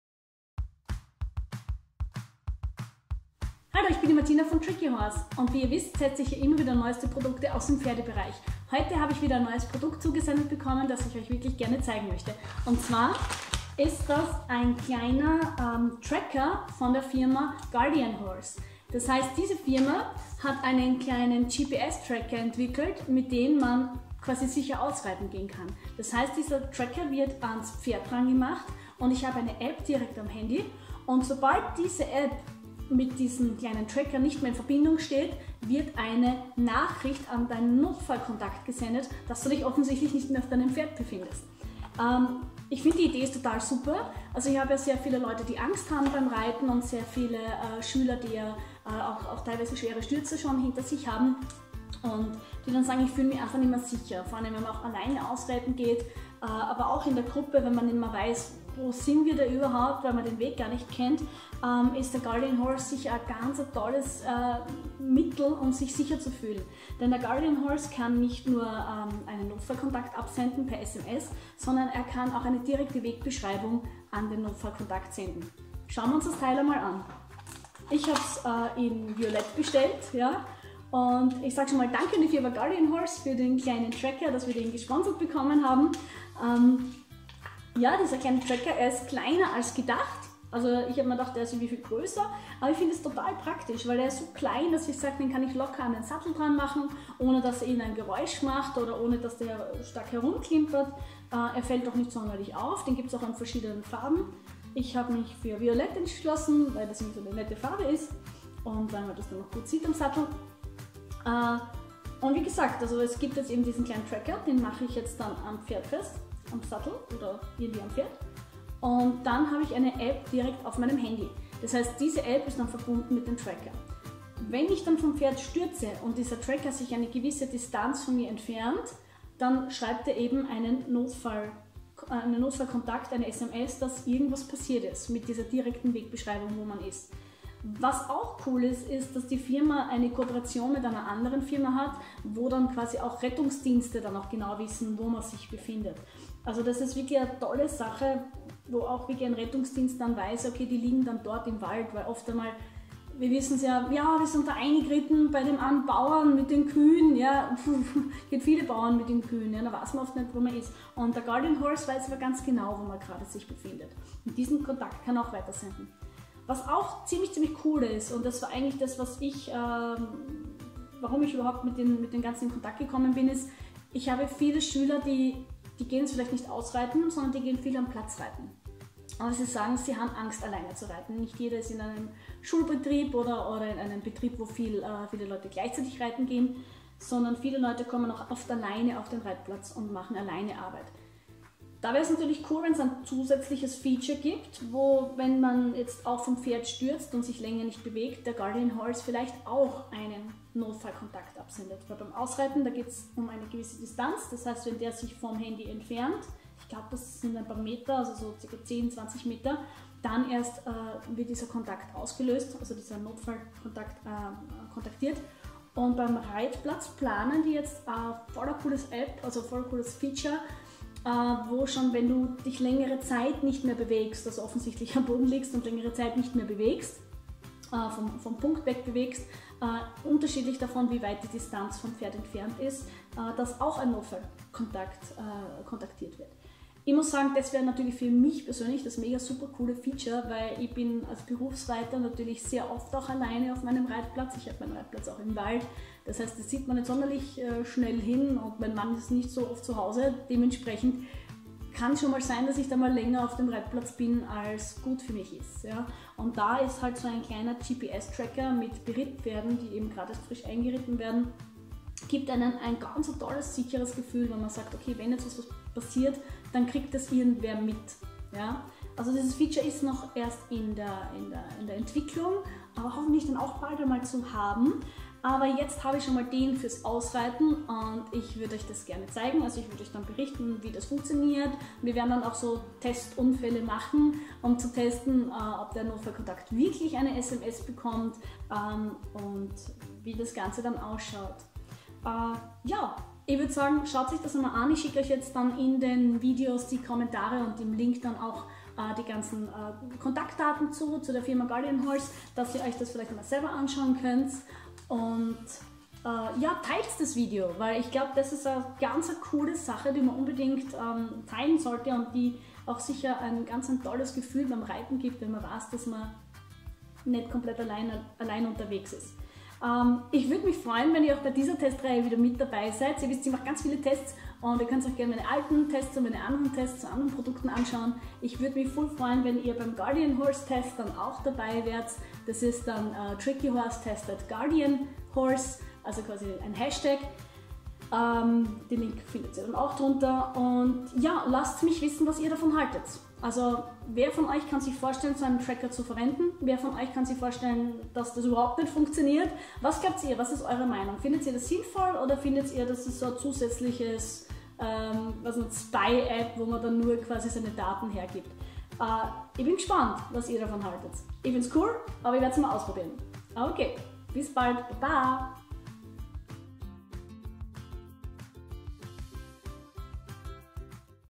Nie wieder Angst beim Ausreiten - Guardian Horse im TrickyHorse Test
🆕NEUE PRODUKT TEST REIHE bei TrickyHorse
-- Guardian Horse - Sicher Ausreiten --🐴

Wie ihr wisst, testen wir immer wieder neue Produkte aus dem Pferdesport.

Der Guardian Horse - sicher Ausreiten Tracker wird beim Reiten im Gelände am Pferd befestigt und mit einer App am Handy verbunden.
Verliert das Pferd seinen Reiter und entfernt sich von ihm, so reißt die Verbindung zum Tracker ab. Sofort bekommt dein Notfallkontakt eine Benachrichtigung über deinen Stürz und erhält die Koordinaten deines aktuellen Standorts! Tolle Sache!

Es wird wohl gerade an einem Zusatzfeature gearbeitet, damit dieses System auch im Viereck funktioniert. Das heißt, die Distanz vom Pferd und Reiter ist dann egal - Sobald man sich länger nicht mehr bewegt, geht das App davon aus, dass man gestürzt ist und gibt wieder eine Sturzwarnung an den Notfallkontakt bekannt. Dieses Feature ist jedoch noch in der Entwicklung :-)

Wie gut der Tracker nun beim Ausreiten funktioniert und wie verlässlich das ganze auch im Wald ist, werden wir die nächsten Wochen wieder ausgiebig mir vielen Videos für Euch testen!

Ich würde mich wieder freuen, wenn ihr meinen Test fleißig liked, kommentiert und teilt! 👍💬😊
Sagt uns Eure Meinung dazu!